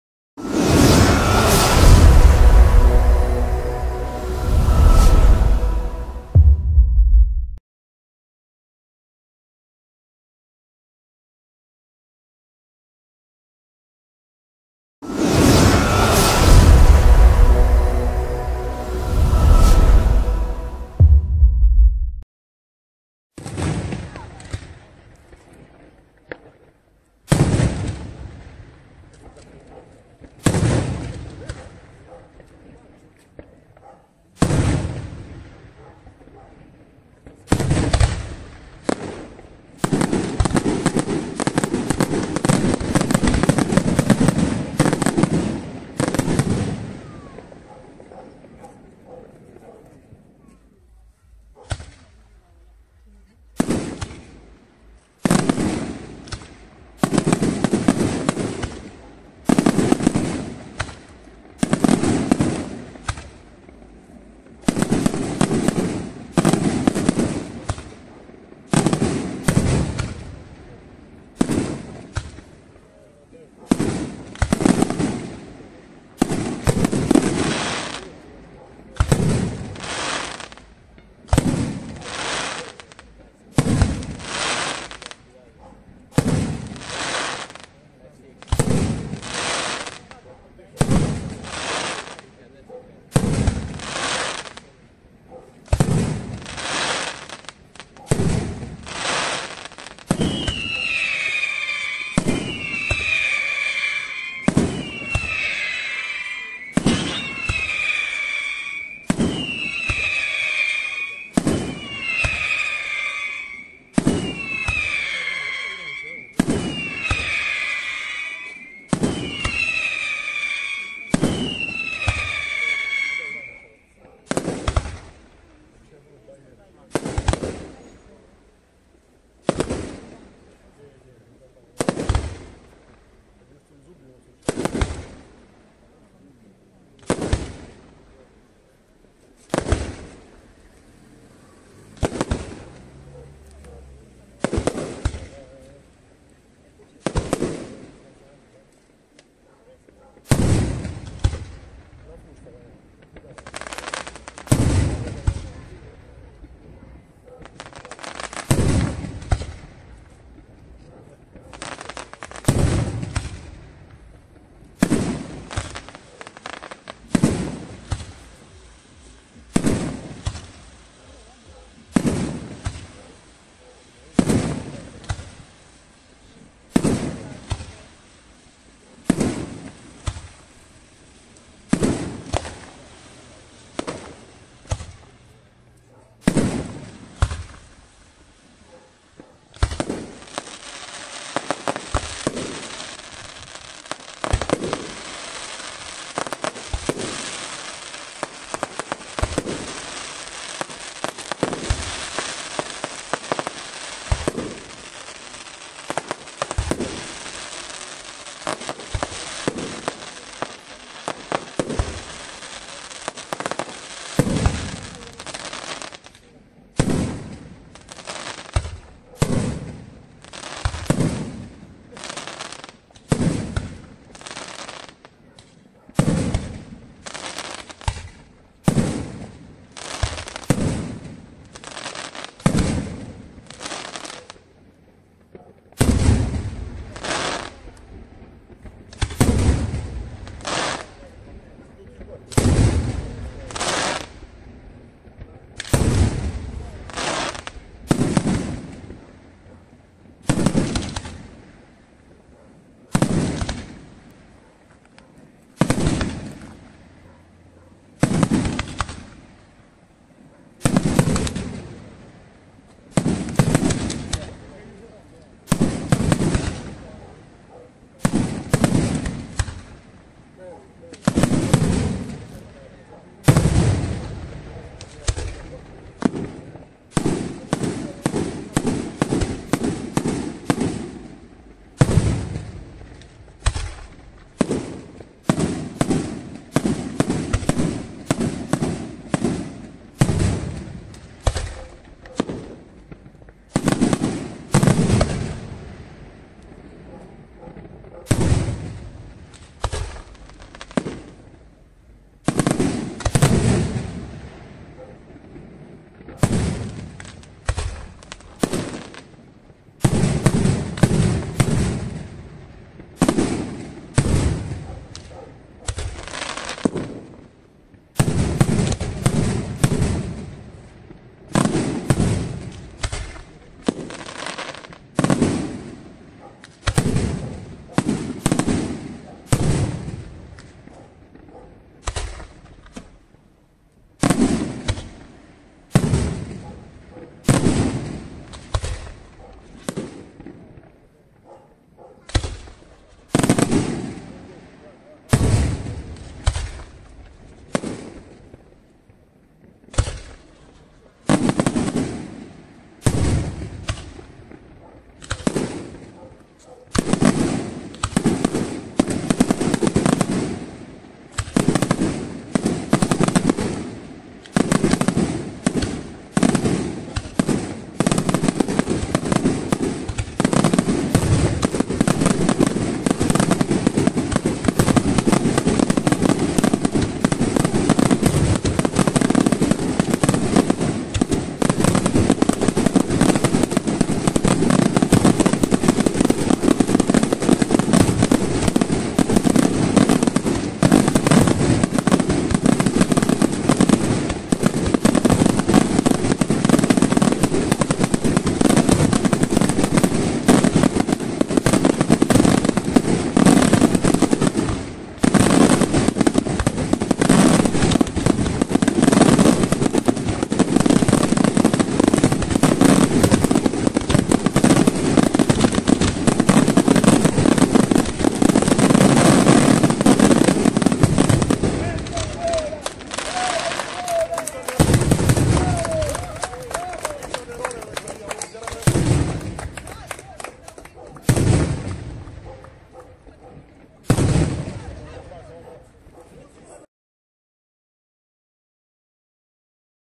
ESTATE 2022 - MARTIGNANO (Le) - San PANTALEONE M. e M. - EMOTION FIREWORKS (Night Show)
Martignano (Le) - 26 Luglio 2022
Festeggiamenti in onore di San Pantaleone Medico e Martire
EMOTION FIREWORKS di Nicola Stasolla da Gioia del Colle (Ba)
Riprese video: Antonio Bodelmonte